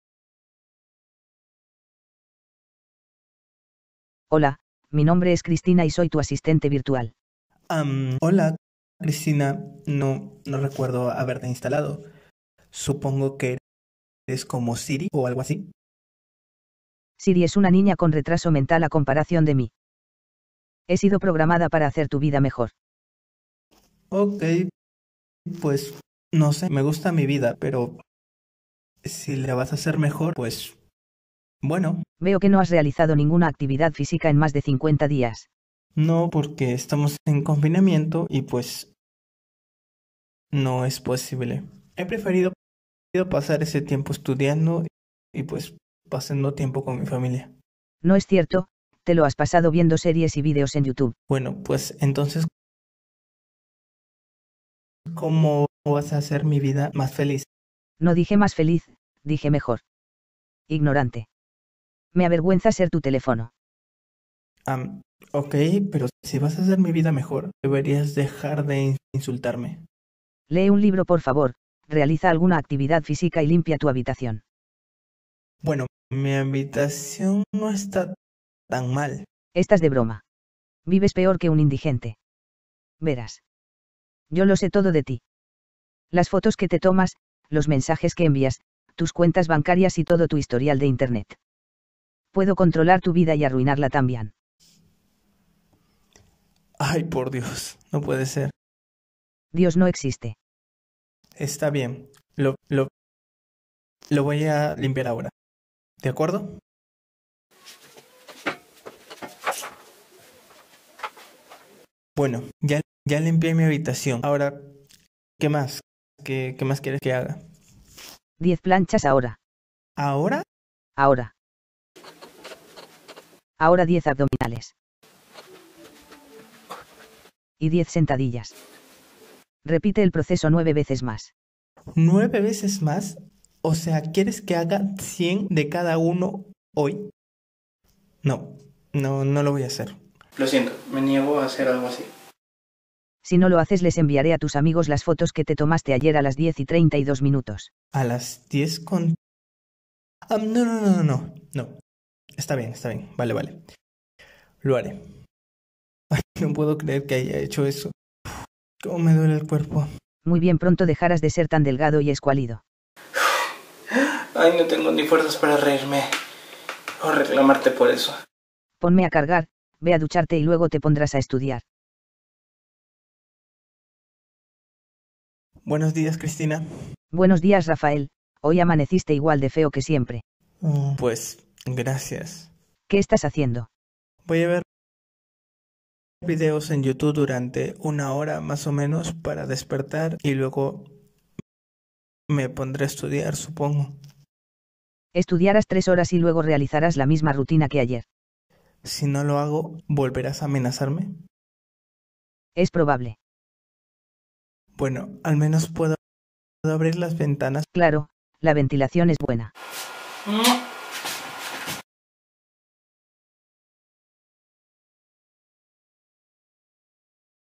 3. Cristina
Centros Juveniles te presenta los cortos que se han presentado al concurso Familias a escena! concurso de cortos en ksa. Esperamos que los disfrutes y que participes a partir del sábado 23 de mayo de 2020 a las 16:00h en la votación formando parte del jurado popular junto con el equipo de Centros Juveniles de Madrid. Para ello rellena este formulario que estará habilitado hasta el próximo miércoles 27 de mayo hasta las 23:55h. ¡Gracias!.